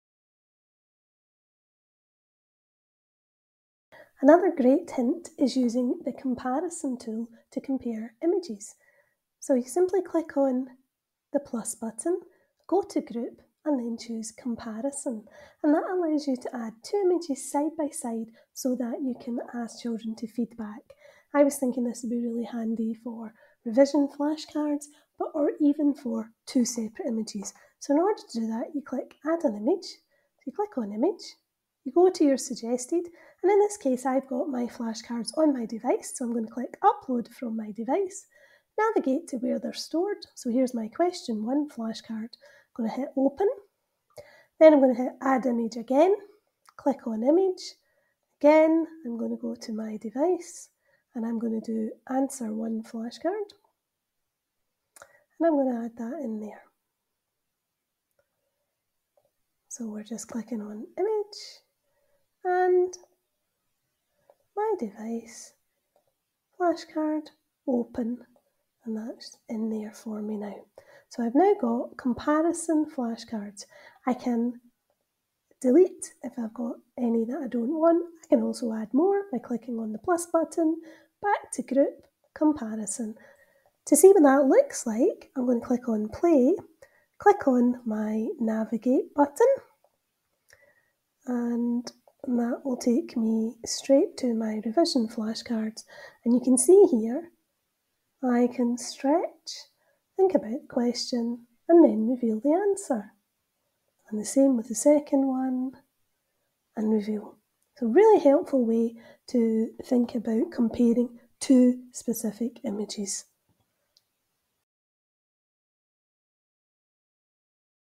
Comparison Images in Sway
This video shows you how to use the Comparison tool in Sway.